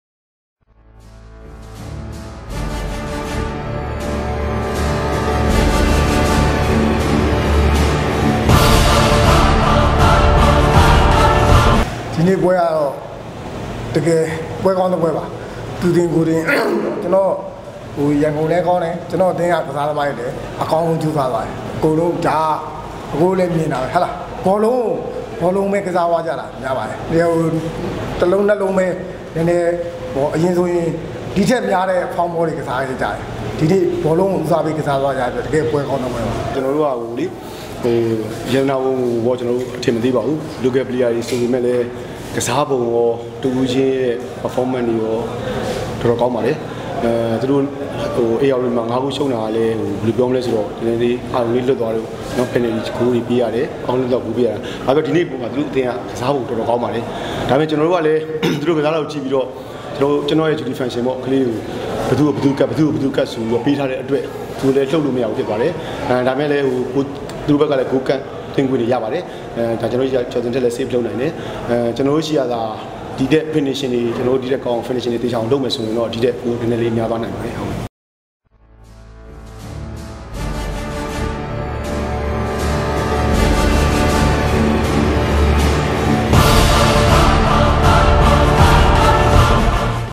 Yangon 1 0 Yadanarbon Head Coach Talk
ရန္ကုန္ယူႏုိက္တက္ ၁-၀ ရတနာပုံအသင္းတုိ႔ပြဲစဥ္အၿပီး နည္းျပႏွစ္ဦးရဲ႕ ပြဲၿပီးေျပာစကားမ်ားကုိ တင္ဆက္ေပးလိုက္ ပါတယ္။